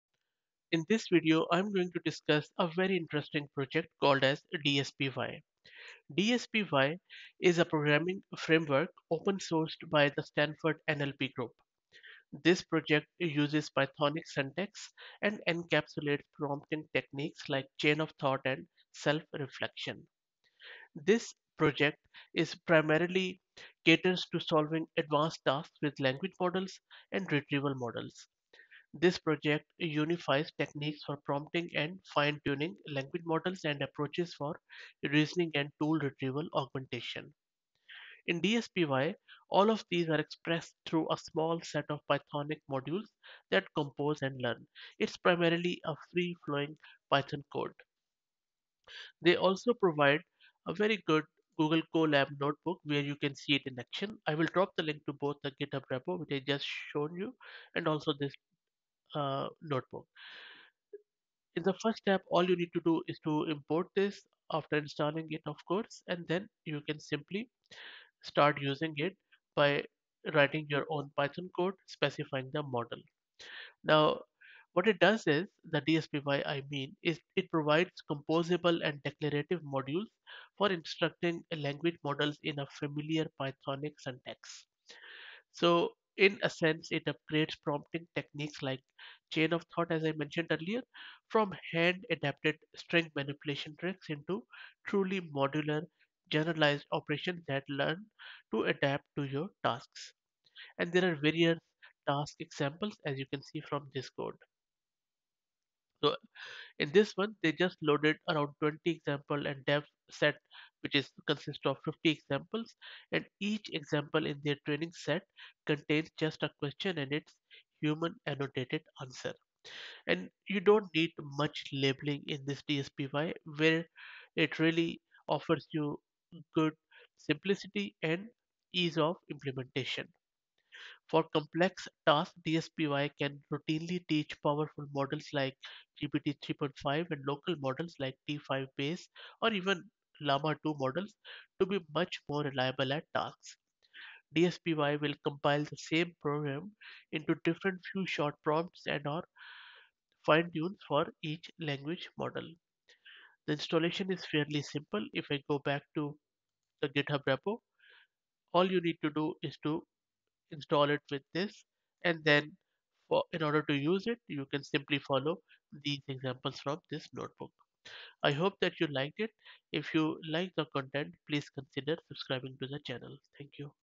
DSPy - Programming with LLMs
This video introduces DSPy. DSPy is the framework for solving advanced tasks with language models (LMs) and retrieval models (RMs).